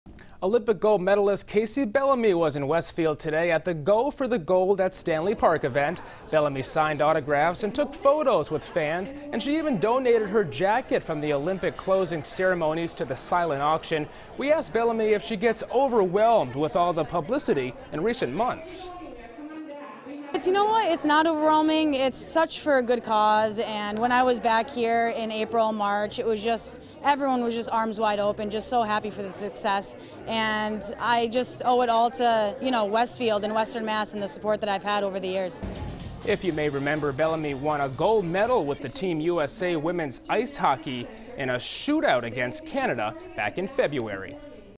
Gold medalist Kacey Bellamy takes part in Westfield "Go for the Gold"
Bellamy signed autographs and took photos, and she even donated her jacket from the Olympic closing ceremonies to the silent auction.